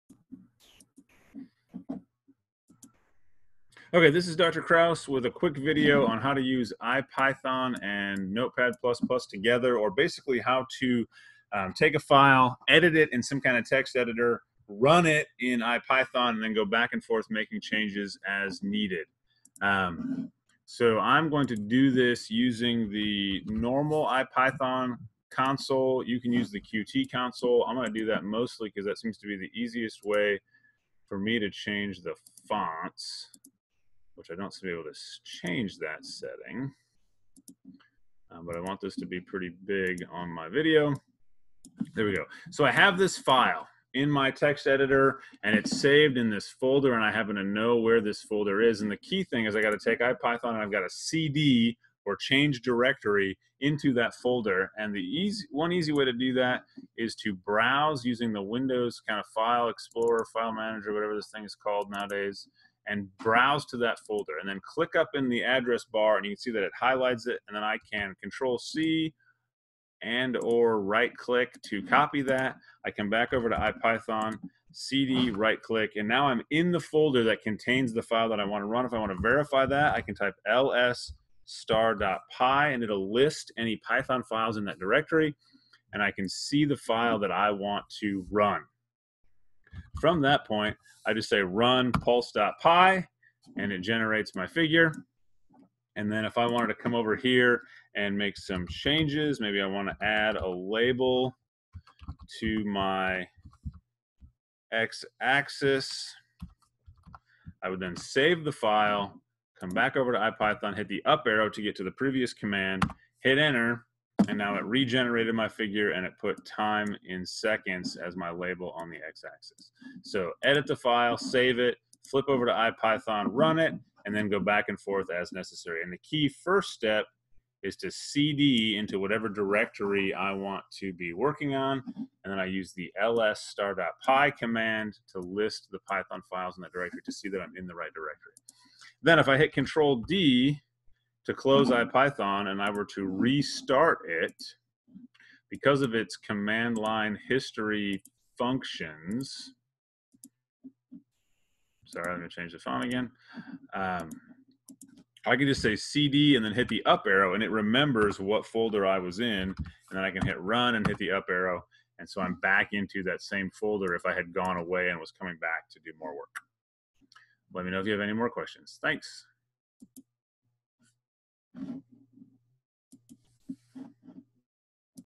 How to run a Python file in IPython
How to execute or run a file in IPython console